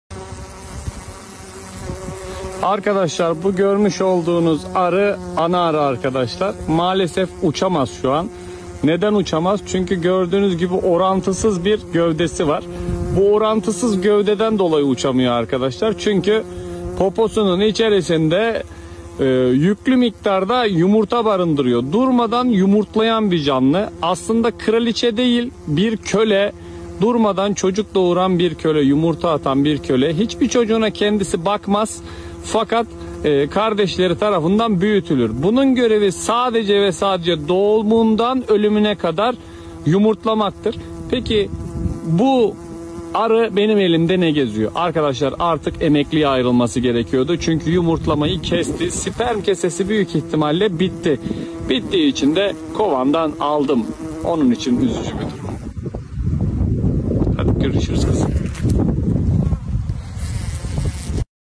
SİZCE... Kraliçe mı ? Köle mi ?
Sizce ?
Kraliçe arı tek yumurta bırakabilen  arıdır. Bir günde 1500 ile 2000 arasında yumurta bırakabilir. Sizce bu arı kraliçe arı mı yoksa köle arı mı ?